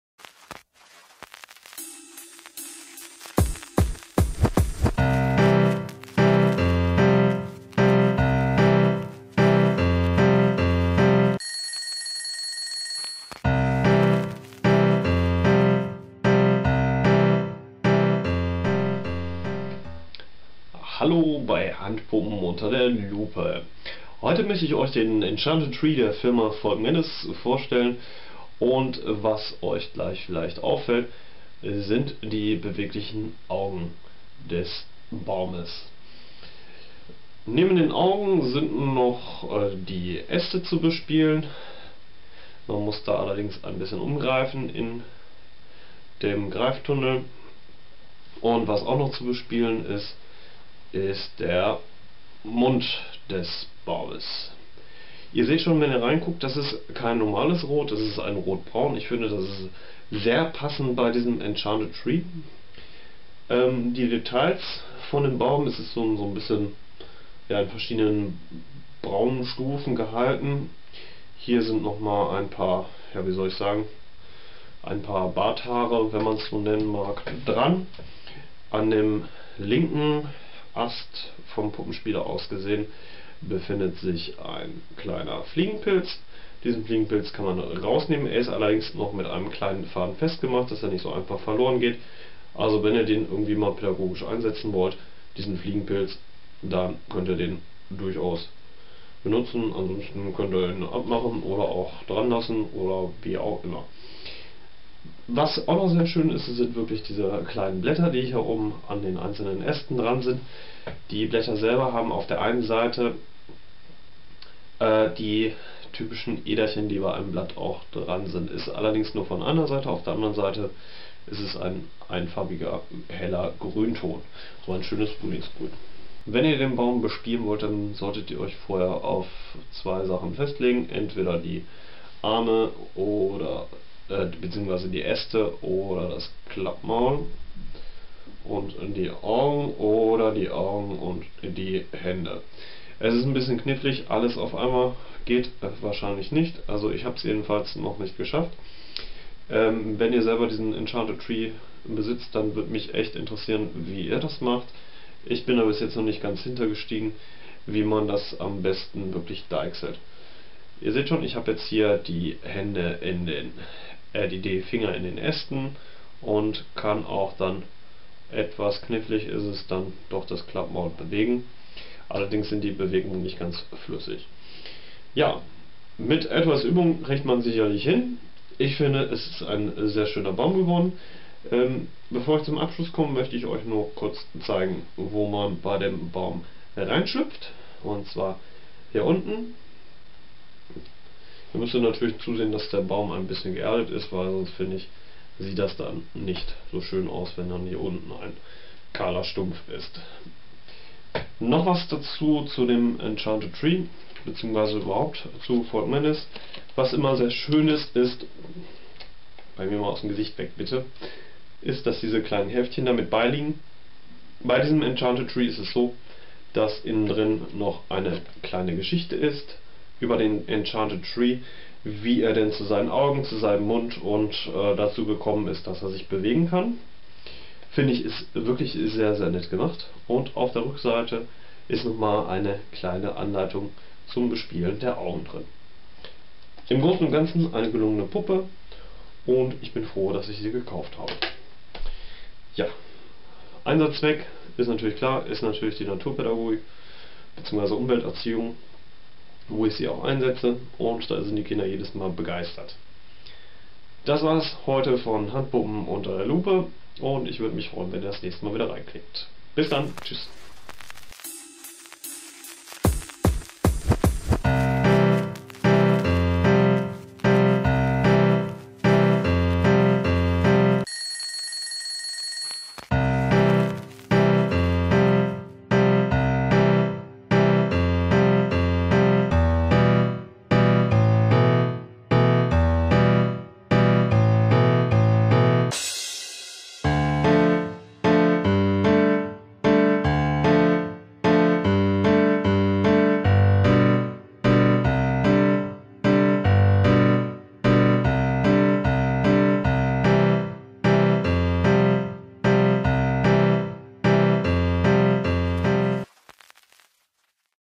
Handpuppen unter der Lupe - Enchanted Tree von Folkmanis
Heute möchte ich euch den "Enchanted Tree" der Firma Folkanis vorstellen. Die Handpuppe gehört zu den wenigen Handpuppen, die bewegliche Augen habe. Neben den Augen sind Mund und Äste bespielbar.

Die Puppe selber ist gut verarbeitet. Die Farben des Baumes sind naturgetreu. Die Oberfläche ist dabei rau gehalten, so dass der typische Look einer Borke entsteht. Einziges Manko ist - so meine bescheidene Meinung - dass sich alle drei Funktionen schwerlich bedienen lassen.

Der Preis liegt bei ca.65 Euro
Produktname: Enchanted Tree
Hersteller: Folkmanis
Größe: 38cm
Preis: 68 Euro